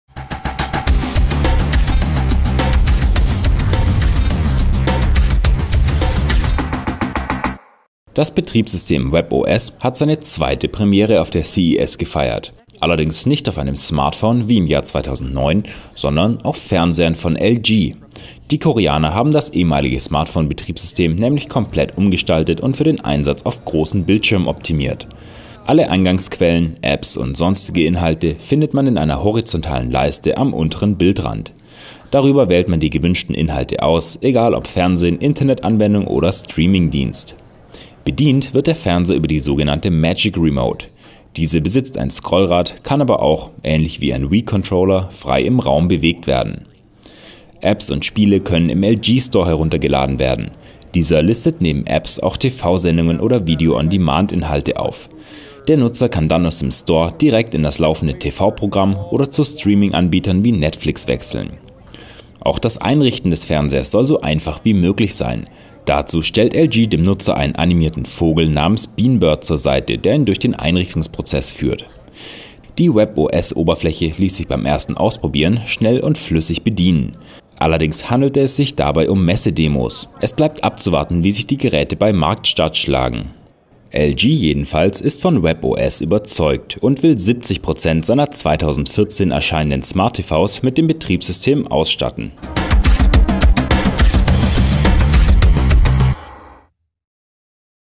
LG stellt Smart-TVs mit webOS-Betriebssystem vor - CES 2014 | deutsch / german
LG hat auf der CES Fernseher mit dem ehemaligen Smartphone-Betriebssystem webOS vorgestellt. Es soll die Bedienung von Smart-TVs vereinfachen und die Auswahl verschiedener Inhalte aus unterschiedlichen Quellen ermöglichen. Einen ersten Eindruck der webOS-Oberfläche erhaltet Ihr in diesem Video.